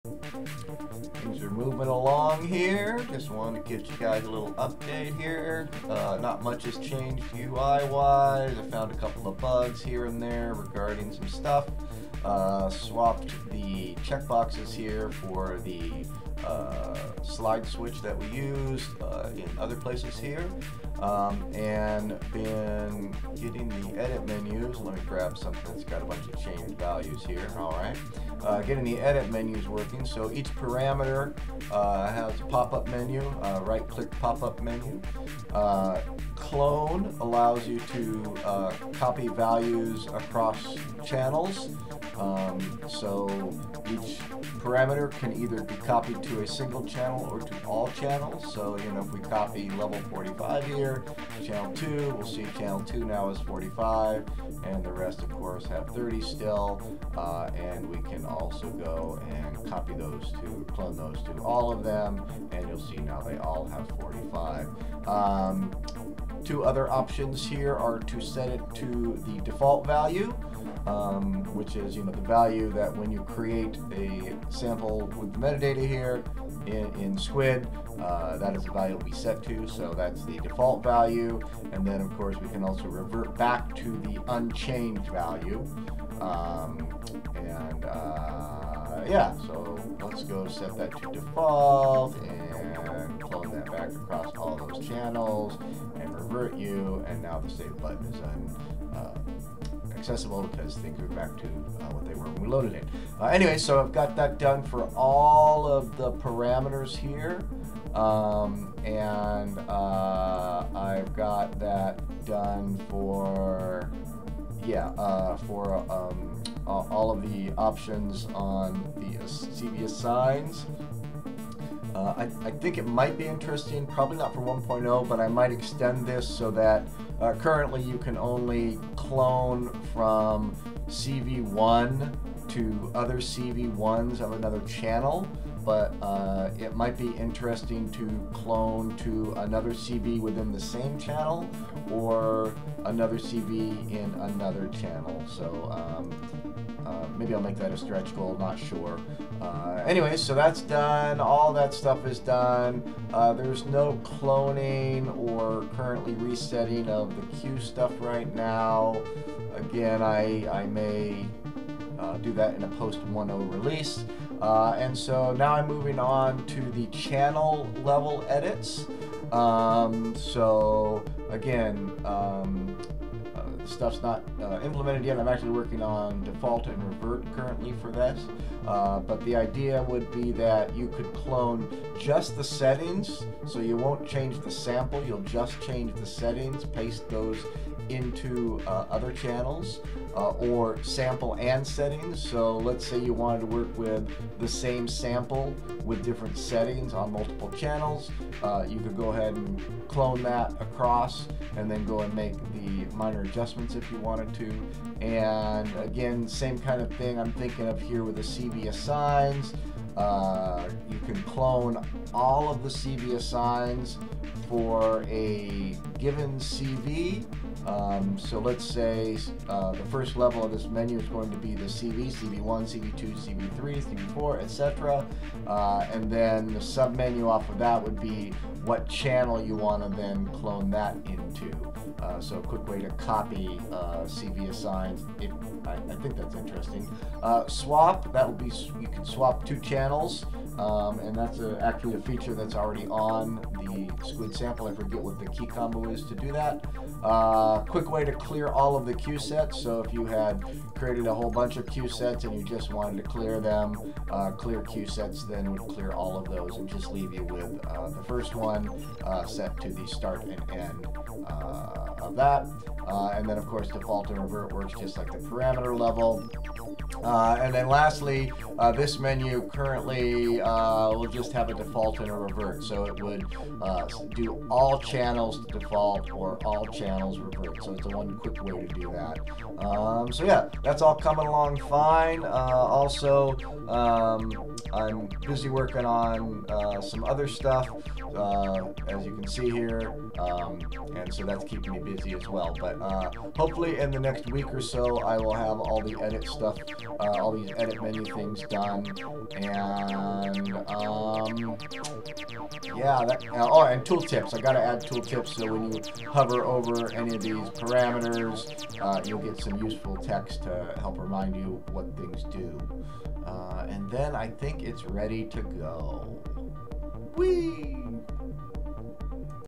Squid Salmple Preset Editor - Development Teaser #17
Implemented the Edit Menus for all Parameters. Working on Channel Edit Menu.